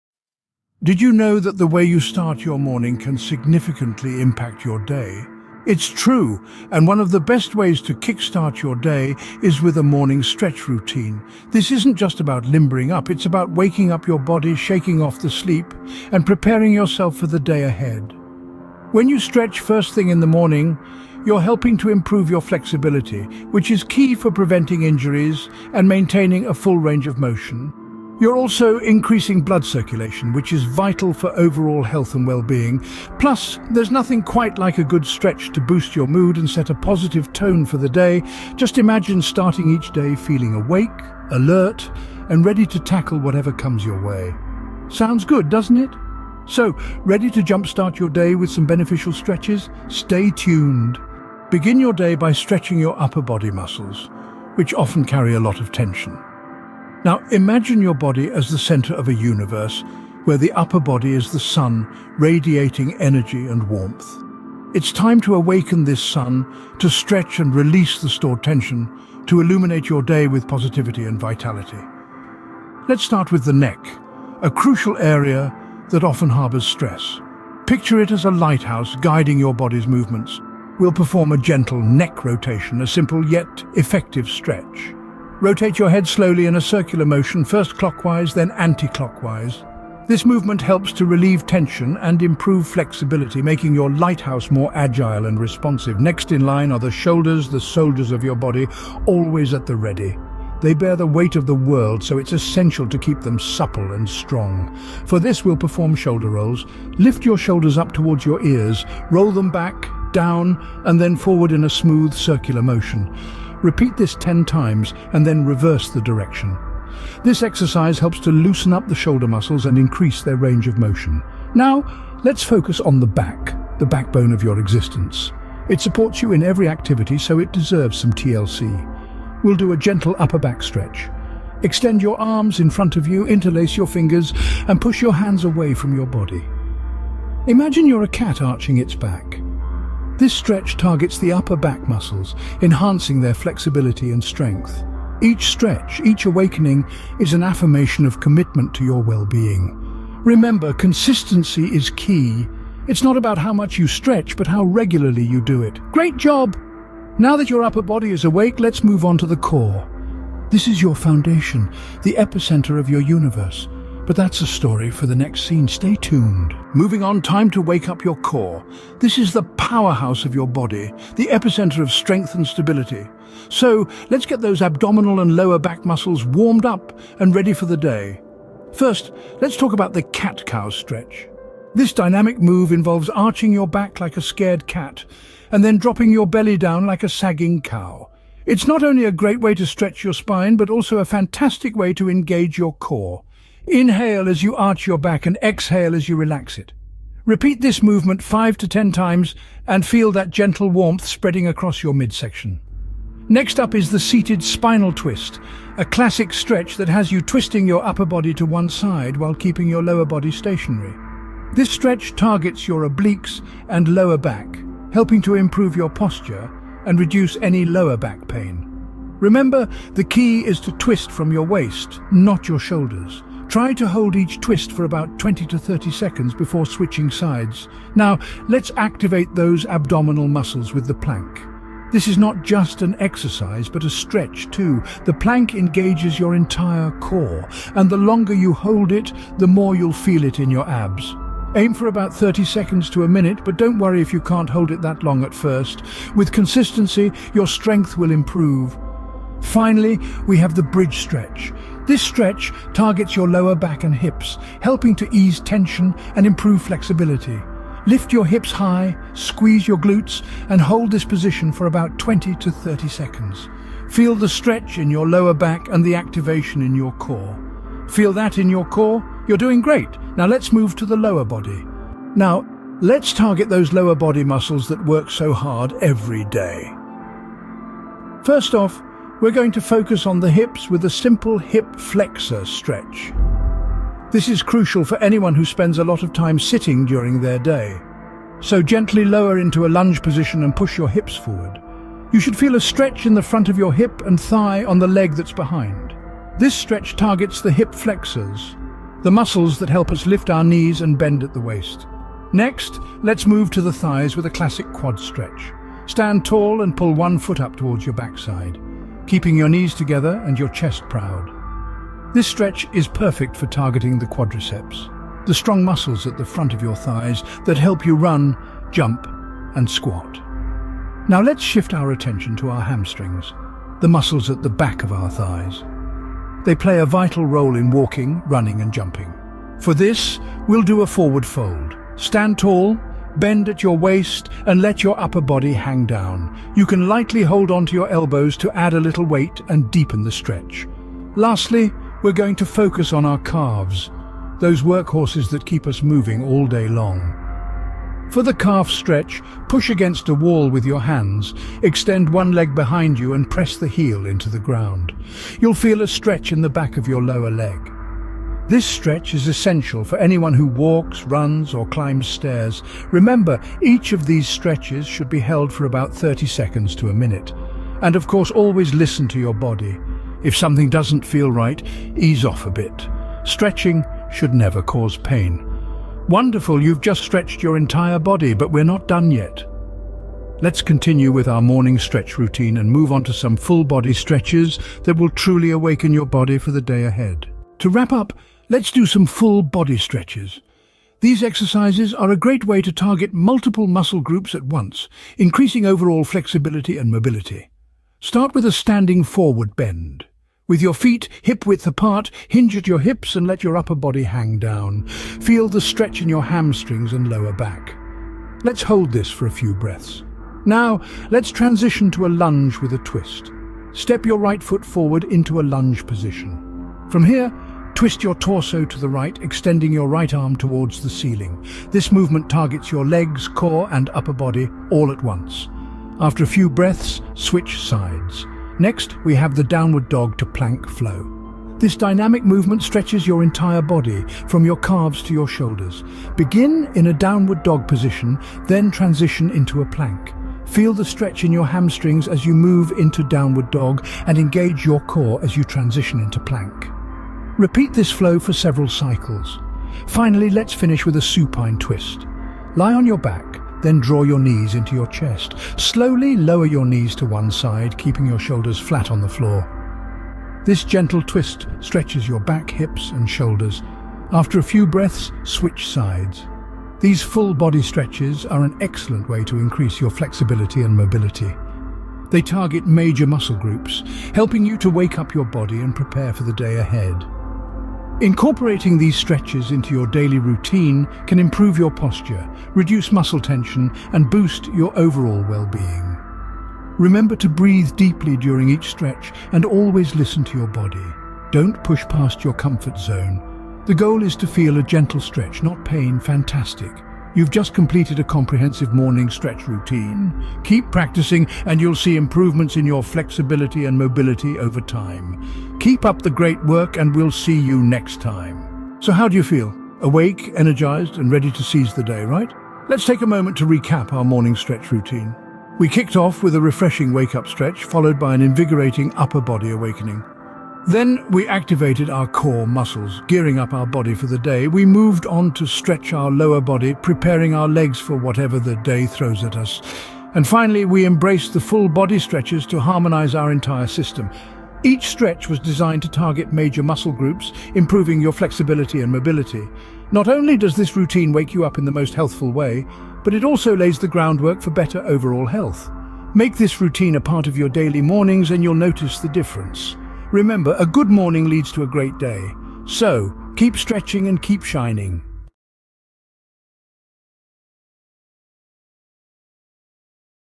10-Minute Morning Stretch Routine for Flexibility
Jumpstart your day with a quick 10-minute morning stretch routine for flexibility.
Morning, stretch, routine, flexibility, energize
Wake Up & Stretch: Your 10-Minute Morning Routine